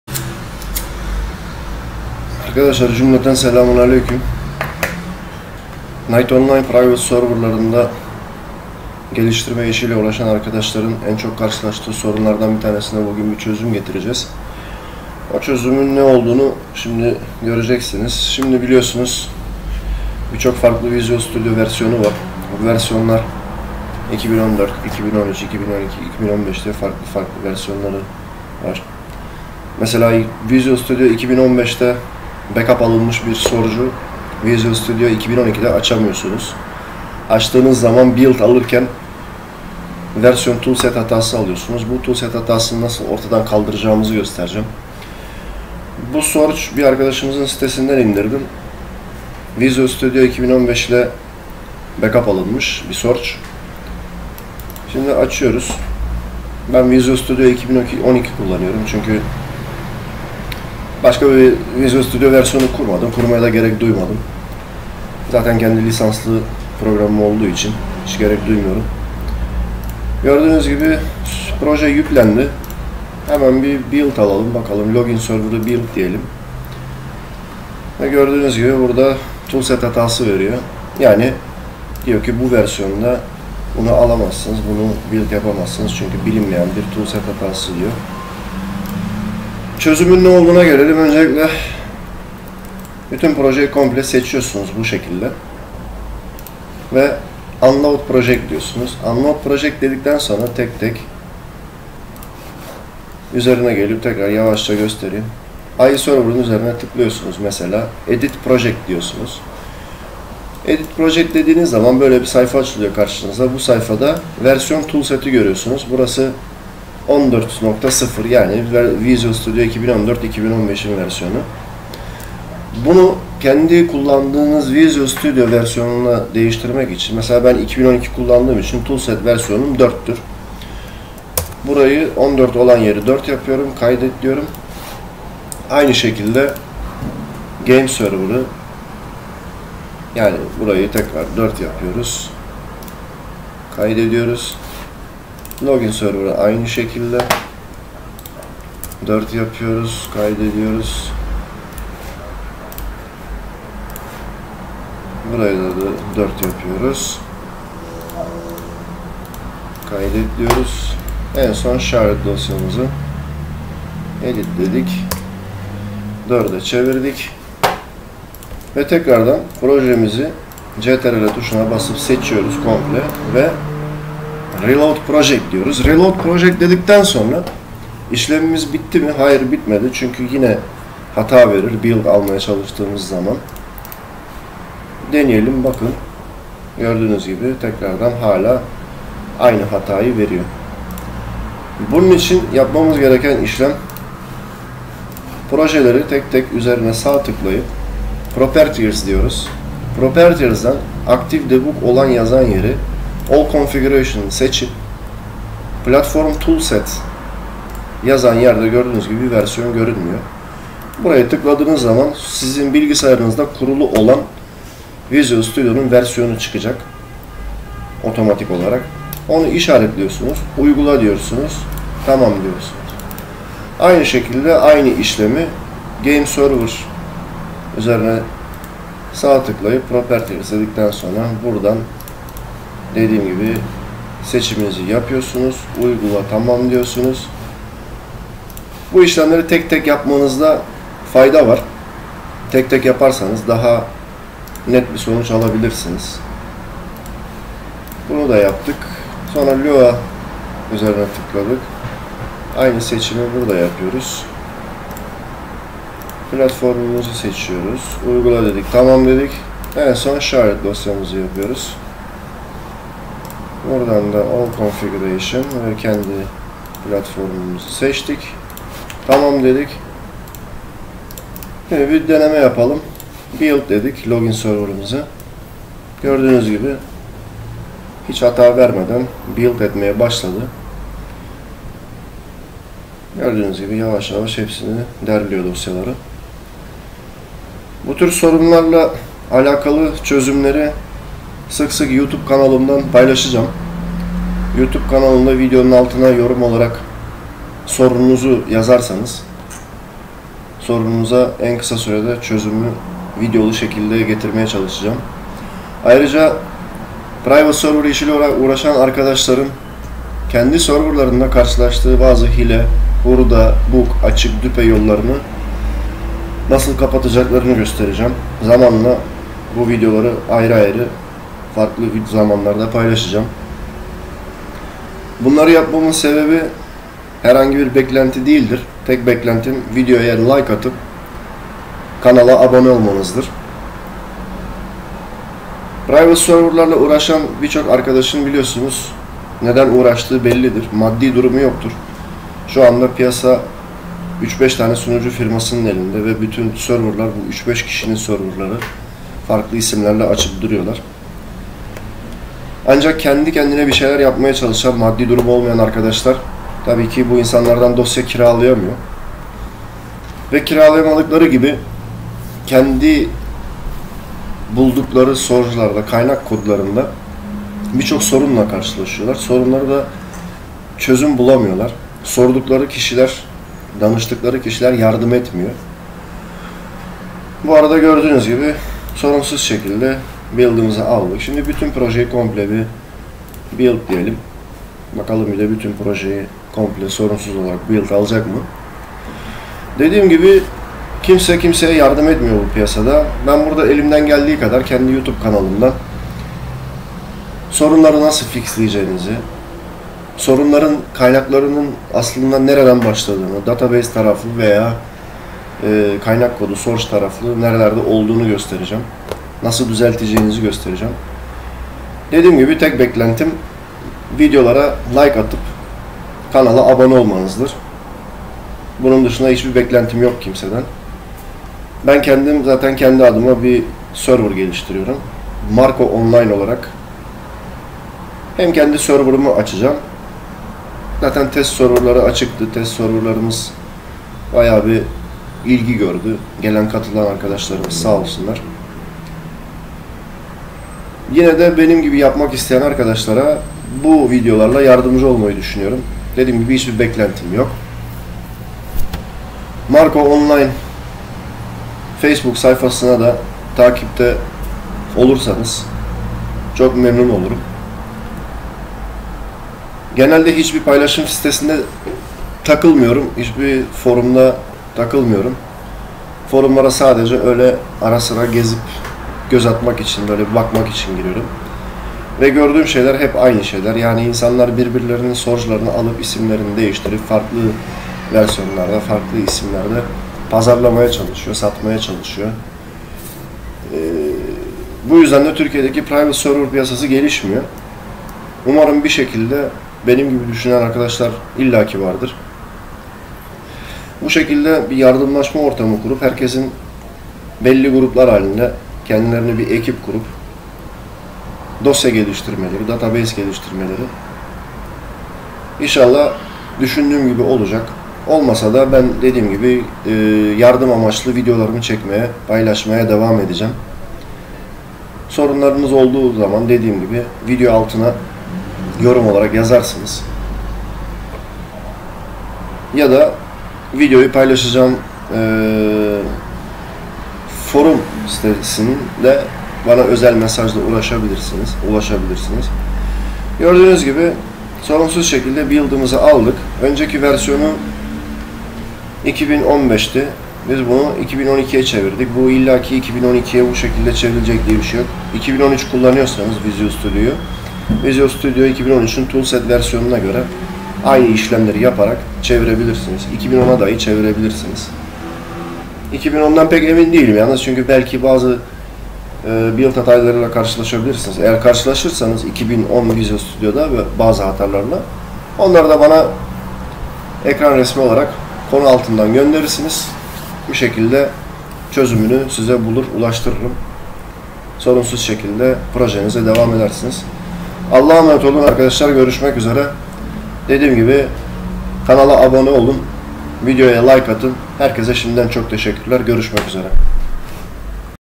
Mar KO Online Visual Studio 2015 Projeyi Visual Studio 2012 ye Cevime (TOOL SET VERSION DEGISTIRME)
Visual studio 2015 back up lı bir kaynak kodunu visual studio 2012 de nasıl sorunsuzca build (derleme) yapabiliriz